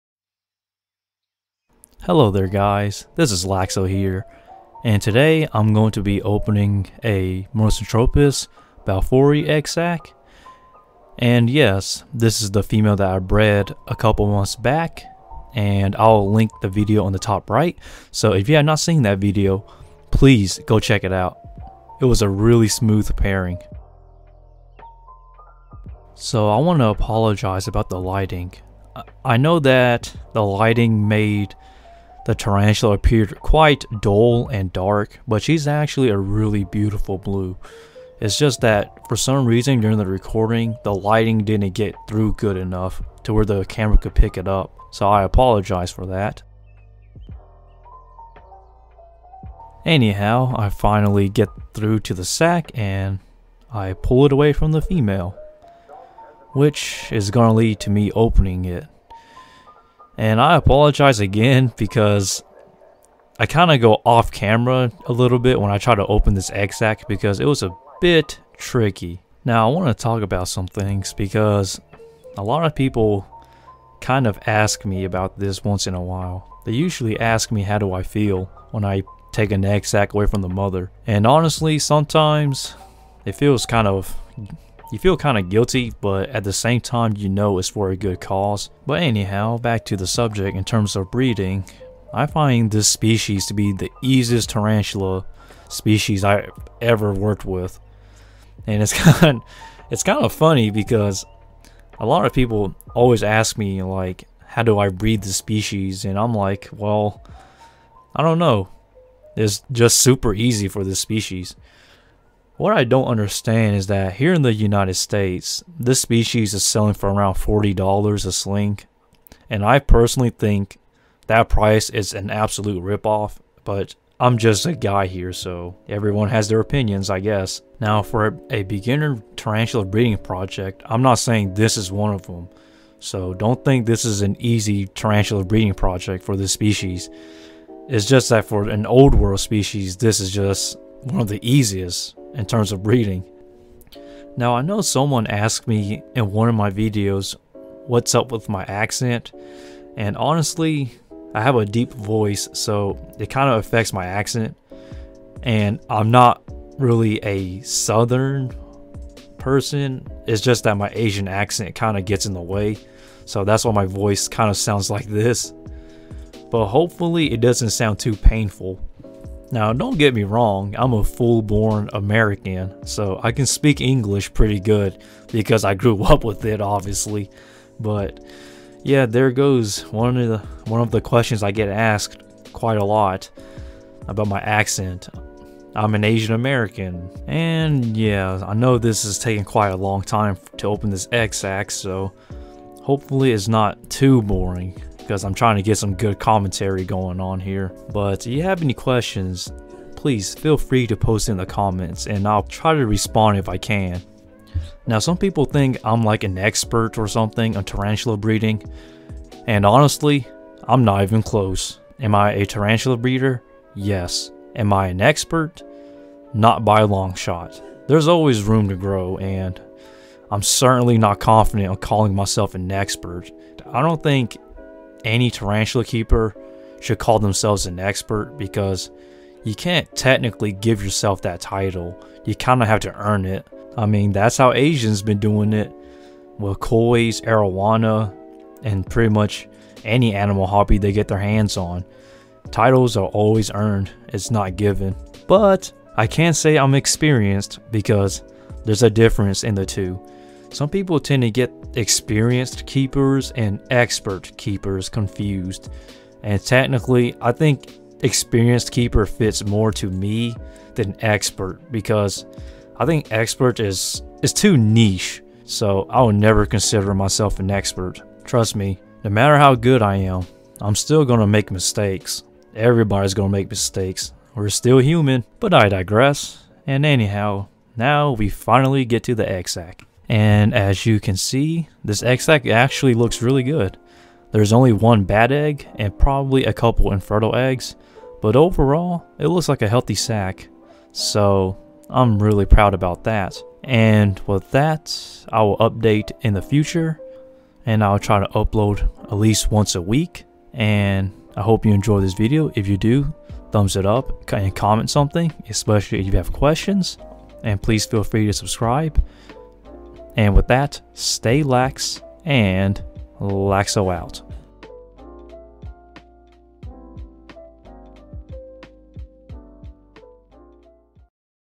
Monocentropus balfouri Tarantula Egg Sac | Tarantula Breeder's Diary
Today I'm pulling and opening the Monocentropus balfouri eggsac, aka. The Socotra Island Blue Leg Baboon Tarantula. I'm trying to work on some of the commentary so there isn't just blank music throughout a video. I hope you viewers prefer more of this kind of stuff... Anyhow, I'm trying to at least get one video a week to see how it goes, but no promises on this schedule. I hope you enjoy this amazing tarantula egg sac pulling and opening.

Species: Monocentropus balfouri | Socotra Island Blue Leg Baboon Tarantula